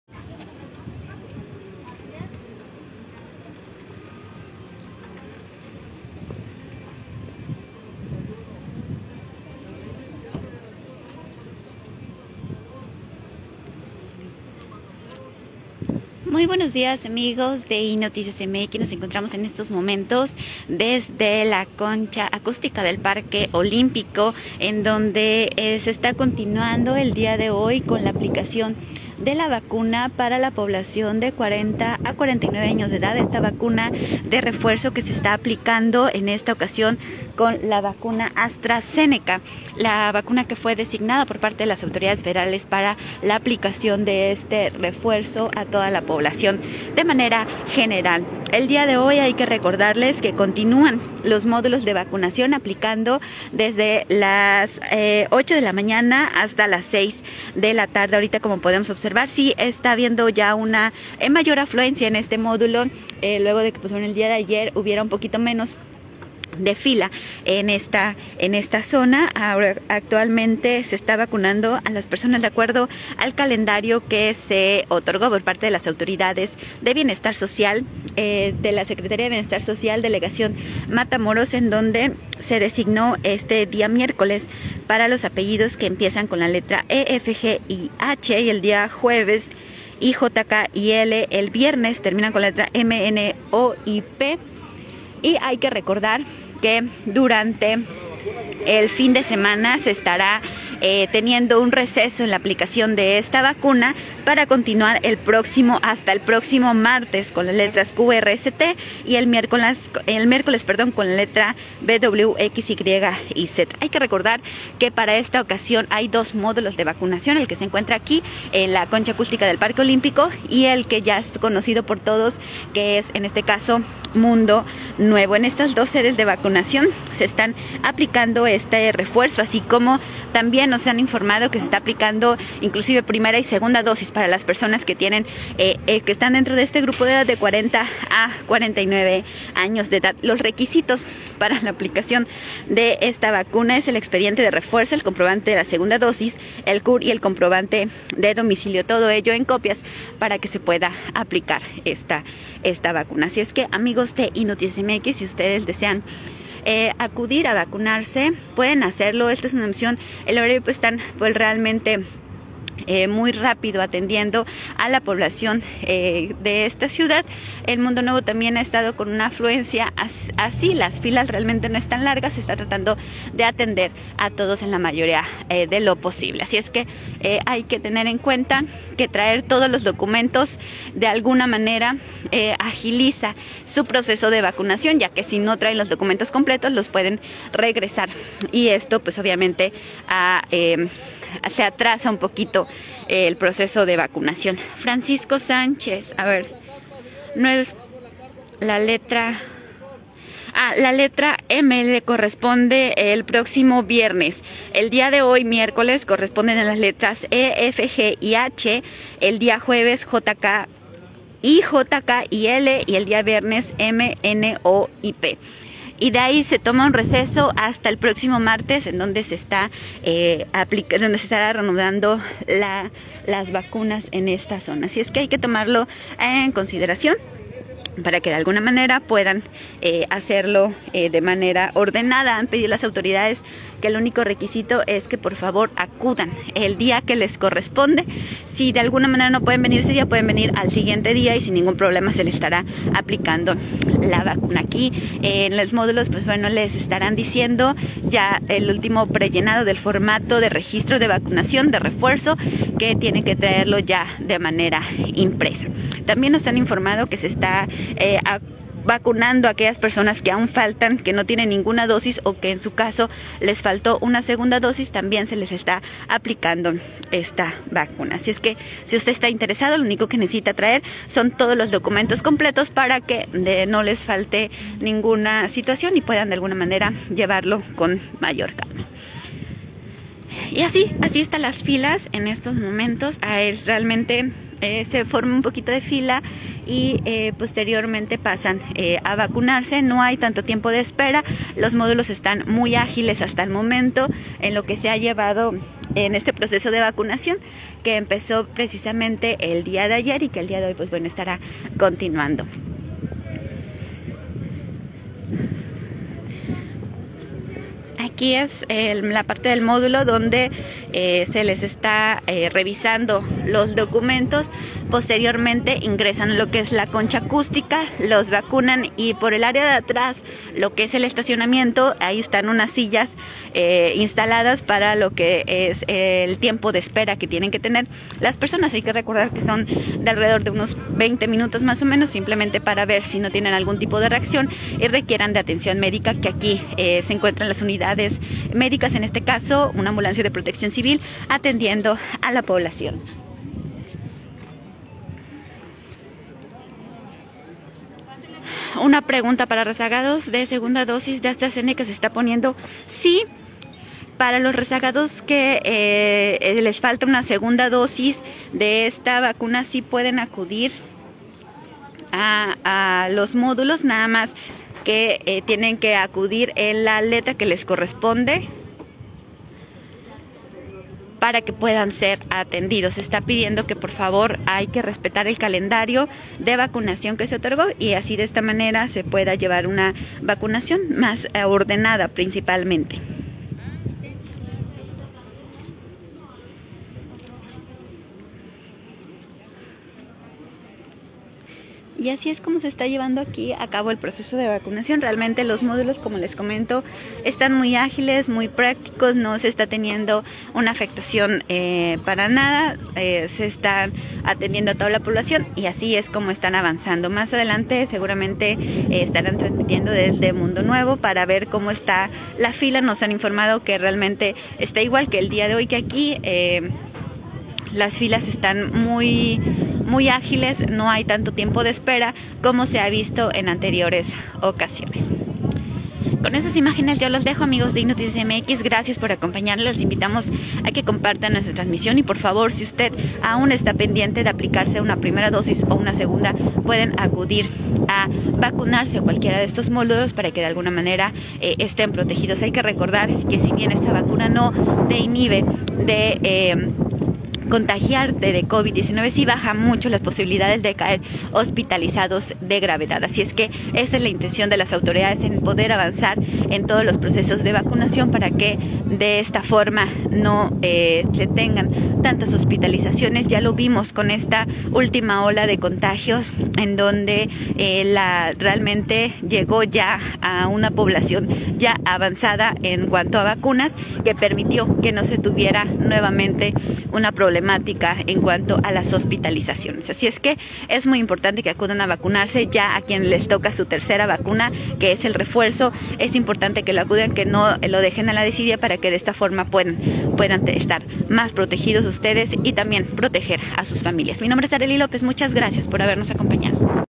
Así el segundo día de vacunación de refuerzo en Matamoros
Este miércoles se realiza el segundo día de vacunación de refuerzo en Matamoros en dos sedes, el parque olímpico y el Centro de Convenciones Mundo Nuevo.